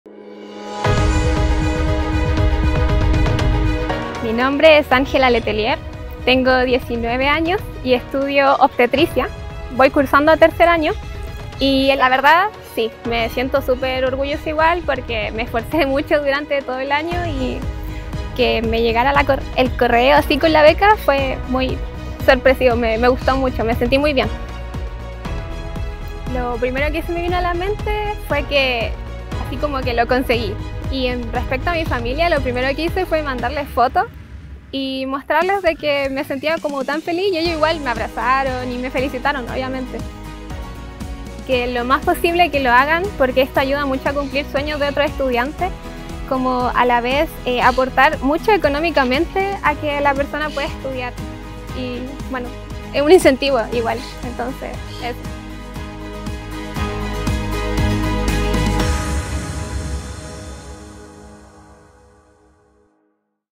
Testimonio Ángela Letelier, Becada Endowment UNAB
Ángela es estudiante de Obstetricia y fue una de las primeras beneficiadas con la Beca de Excelencia Académica otorgada por el Endowment UNAB. Conoce a continuación su testimonio.